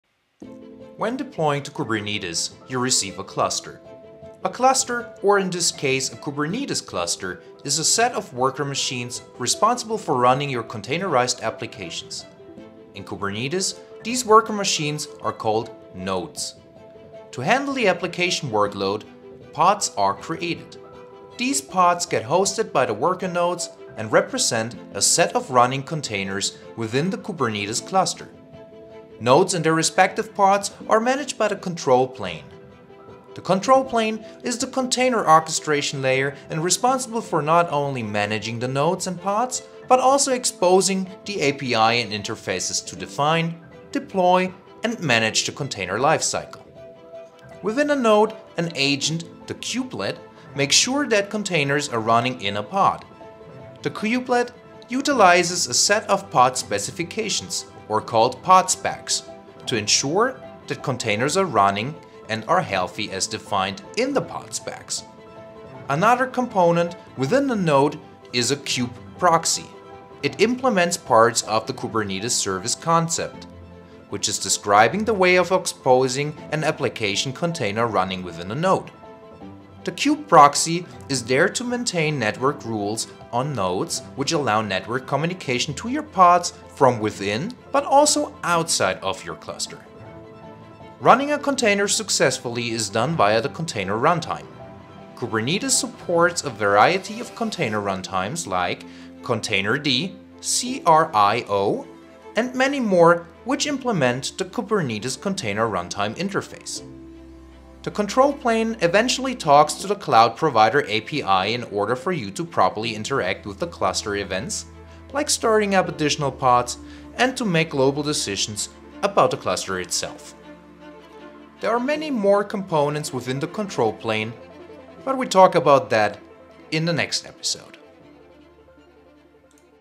Cloud Native for Beginners: The Kubernetes Components
In this fifth episode for beginners of "2 Minutes Of Cloud Native" you will get a high-level architectural view on the different Kubernetes components! Make sure to check out the subsequent episodes.